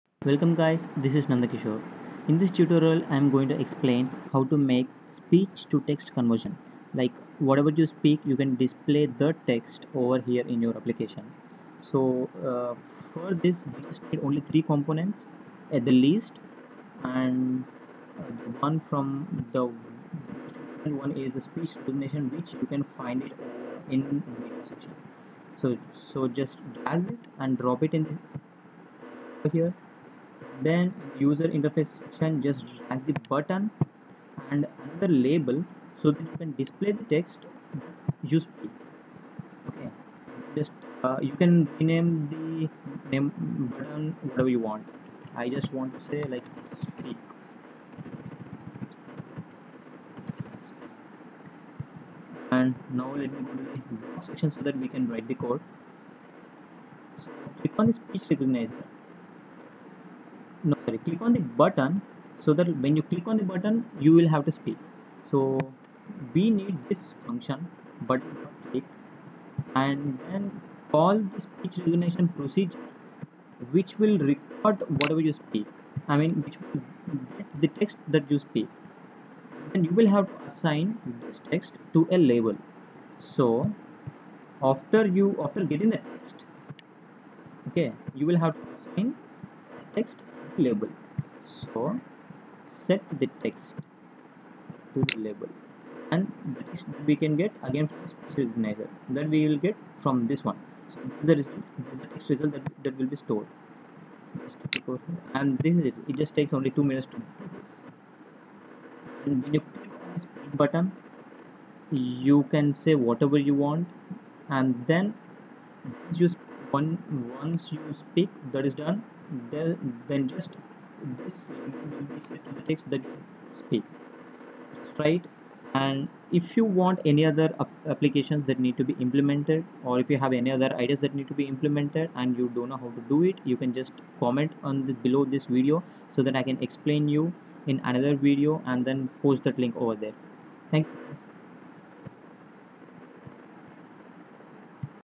Speech to Text Conversion using MIT App Inventor 2
This video shows you how to make convert your speech to text and display it in your app. You can use this as whole application for some purpose or may be as part of your application.